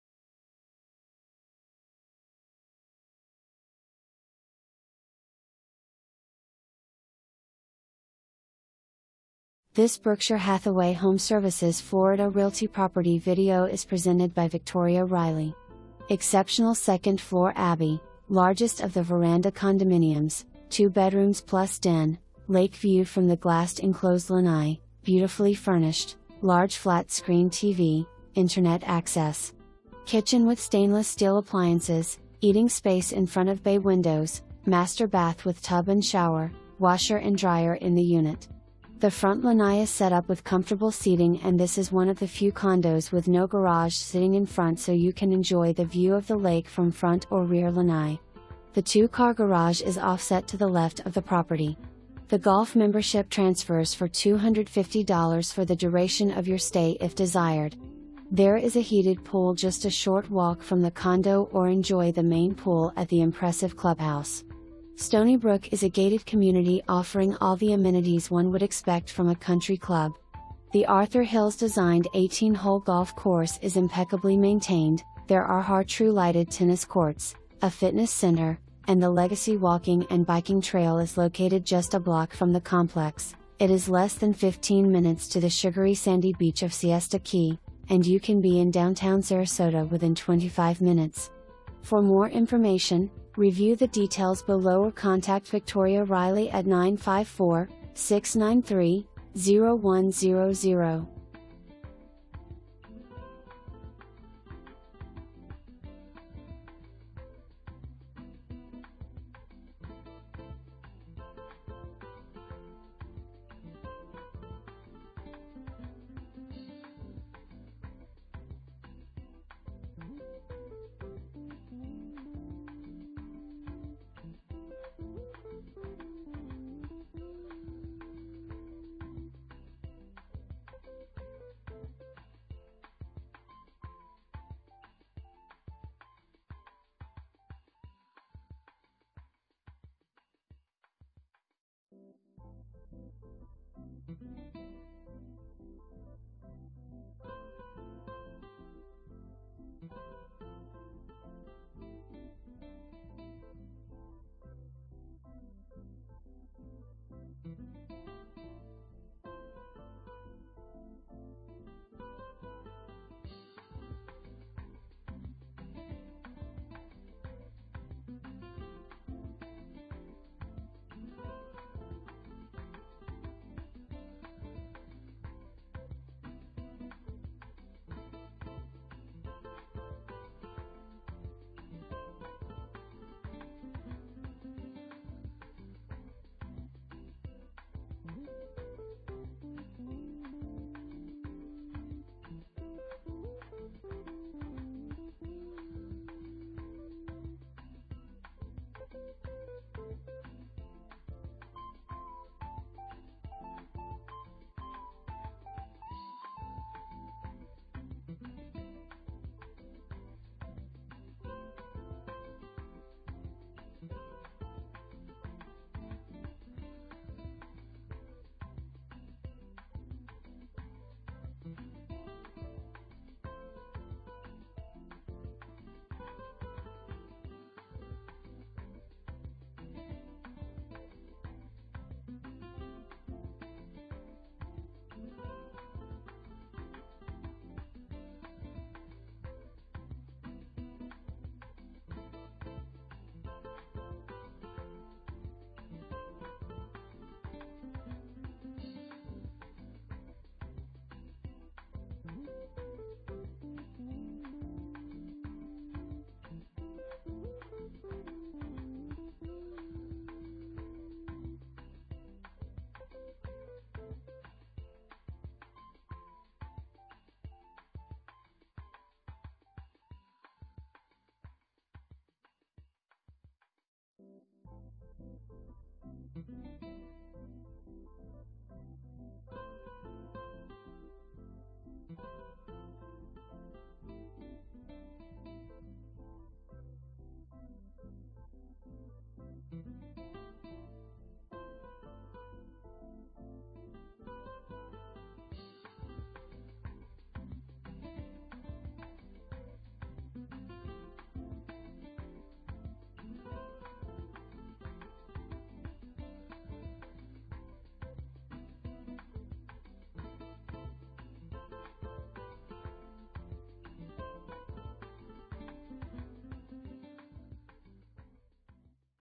Berkshire Hathaway HomeServices Florida Realty - 9601 CASTLE POINT DRIVE #821
9601 CASTLE POINT DRIVE #821

Exceptional 2nd floor Abbey, largest of the Veranda Condominiums, two bedrooms plus den, lake view from the glassed enclosed lanai, beautifully furnished, large flat-screen TV, Internet access. Kitchen with SS appliances, eating space in front of bay windows, master bath with tub and shower, washer and dryer in the unit. The front lanai is set up with comfortable seating and this is one of the few condos with no garage sitting in front so you can enjoy the view of the lake from front or rear lanai. The two-car garage is offset to the left of the property. The golf membership transfers for $250 for the duration of your stay if desired. There is a heated pool just a short walk from the condo or enjoy the main pool at the impressive clubhouse. Stoneybrook is a gated community offering all the amenities one would expect from a country club. The Arthur Hills designed 18 hole golf course is impeccably maintained, there are Har Tru lighted tennis courts, a fitness center, and the Legacy walking and biking trail is located just a block from the complex, it is less than 15 minutes to the sugary sandy beach of Siesta Key, and you can be in downtown Sarasota within 25 minutes.

Victoria Reilly
Berkshire Hathaway HomeServices Florida Realty